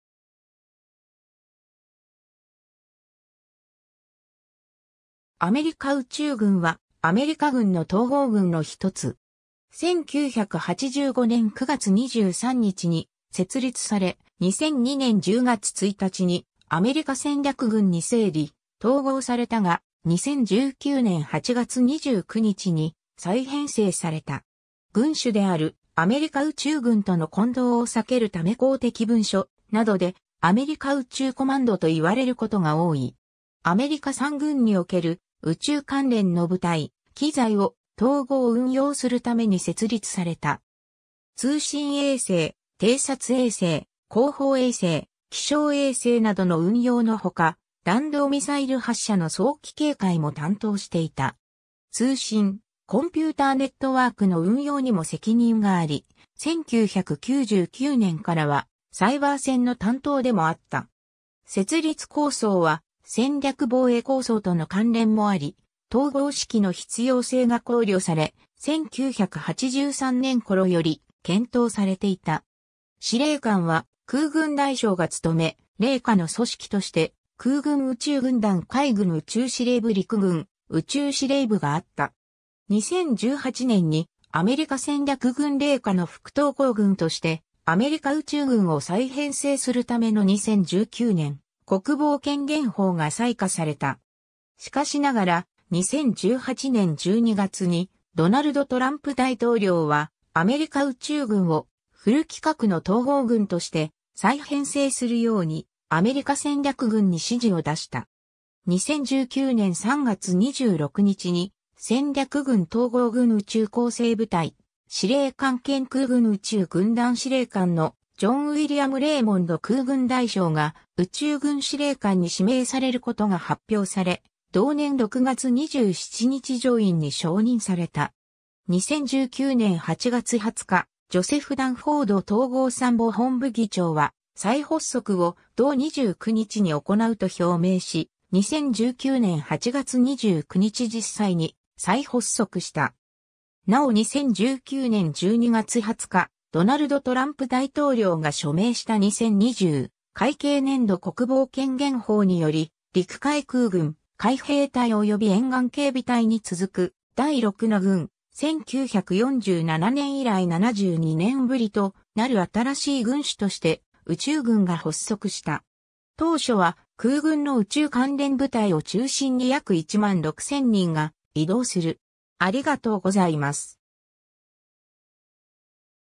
アメリカ宇宙軍 (統合軍)
アメリカ宇宙軍（アメリカうちゅうぐん、United States Space Command：USSPACECOM）は、アメリカ軍の統合軍の一つ。
1985年9月23日に設立され、2002年10月1日にアメリカ戦略軍に整理・統合されたが、2019年8月29日に再編成された。
軍種であるアメリカ宇宙軍との混同を避けるため公的文書などでアメリカ宇宙コマンドと言われることが多い。
 アメリカ三軍における宇宙関連の部隊・機材を統合運用するために設立された。
通信衛星・偵察衛星・航法衛星・気象衛星などの運用のほか、弾道ミサイル発射の早期警戒も担当していた。
通信・コンピューターネットワークの運用にも責任があり、1999年からはサイバー戦の担当でもあった。
設立構想は戦略防衛構想との関連もあり、統合指揮の必要性が考慮され、1983年頃より検討されていた。
 司令官は空軍大将が務め、隷下の組織として、空軍宇宙軍団 (AFSPC) 、海軍宇宙司令部 (NAVSPACECOM) 、陸軍宇宙司令部 (USARSPACE) があった。
 2018年に、アメリカ戦略軍隷下の副統合軍 (sub-unified combatant command) としてアメリカ宇宙軍を再編成するための2019年国防権限法が裁可された。
しかしながら、2018年12月にドナルド・トランプ大統領は、アメリカ宇宙軍をフル規格の統合軍として再編成するように、アメリカ戦略軍に指示を出した。
 2019年3月26日に、戦略軍統合軍宇宙構成部隊司令官兼空軍宇宙軍団 (AFSPC) 司令官のジョン・ウイリアム・レイモンド空軍大将が、宇宙軍司令官に指名されることが発表され、同年6月27日上院に承認された。
2019年8月20日、ジョセフ・ダンフォード統合参謀本部議長は、再発足を同29日に行うと表明し、2019年8月29日実際に再発足した。
 なお2019年12月20日、ドナルド・トランプ大統領が署名した2020会計年度国防権限法により、陸海空軍、海兵隊及び沿岸警備隊に続く「第6の軍」、1947年以来72年ぶりとなる新しい軍種として「宇宙軍」が発足した。
当初は空軍の宇宙関連部隊を中心に約1万6000人が移動する。